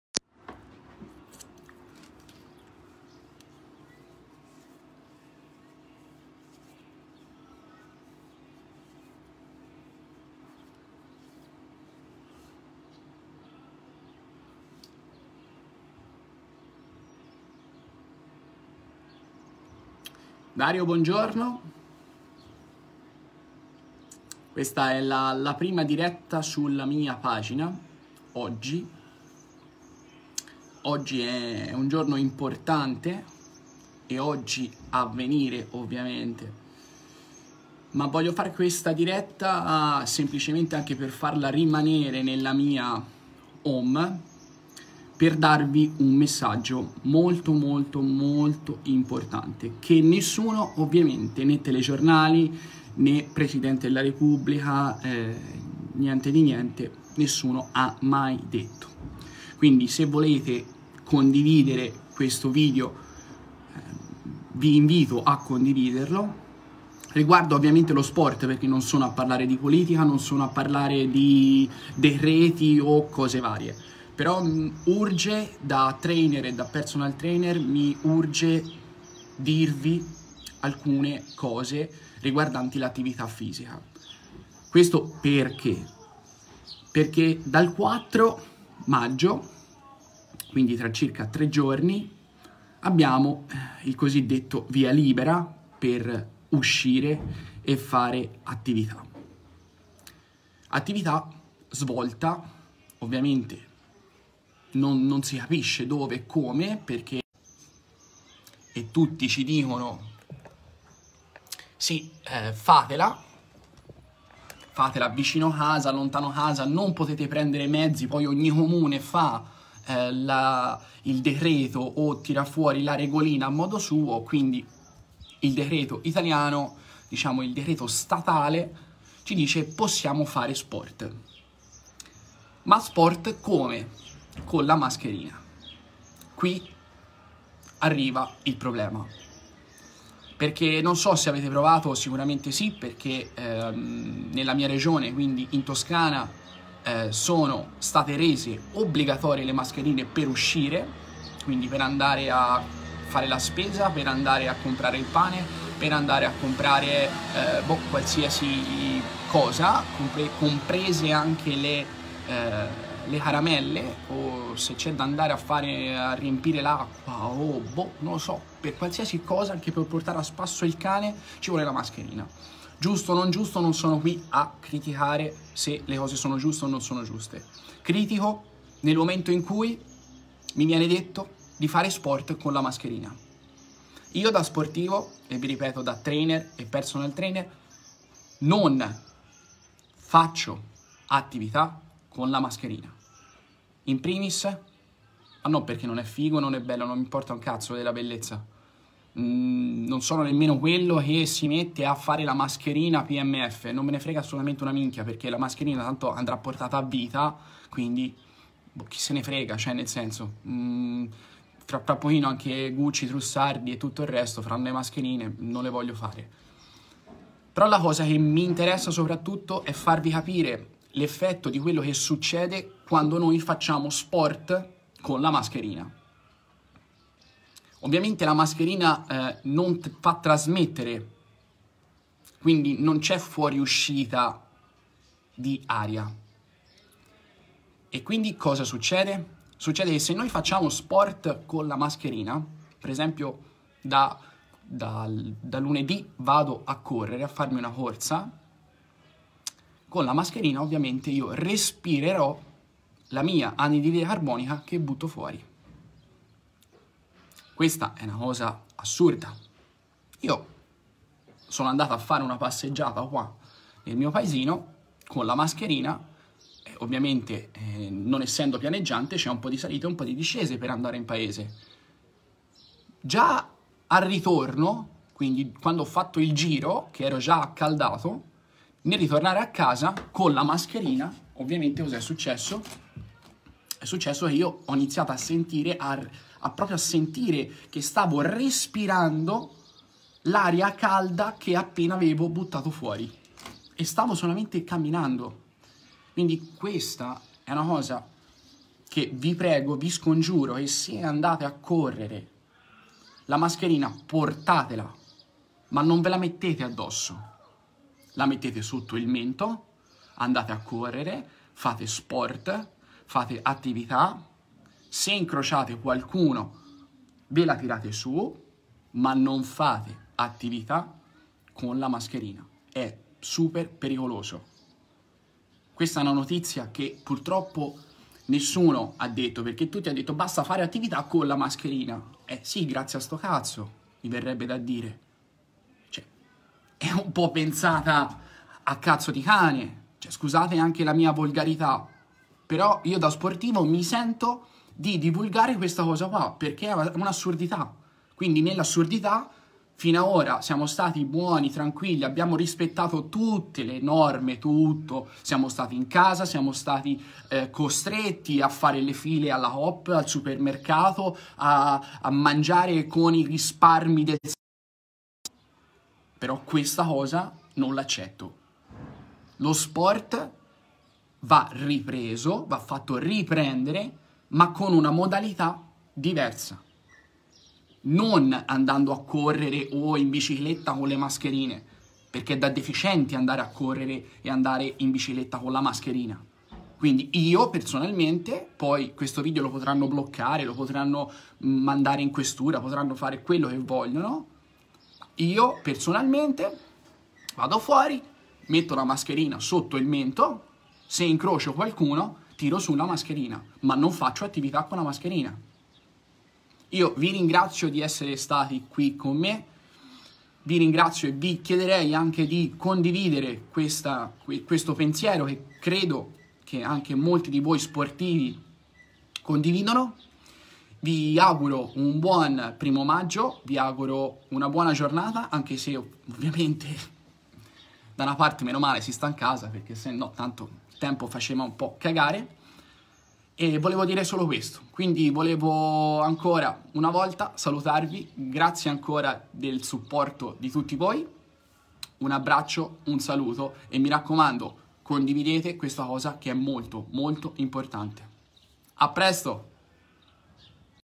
Fare Sport Con le Mascherine !!!??! IDIOZIA
QUESTO è QUELLO CHE NON DICE NESSUNO ... MA DA LUNEDì ...?!?

DA SPORTIVO ... DICO LA MIA NO ALLO SPORT CON LE MASCHERINE !!!

E' PERICOLOSO COVID-19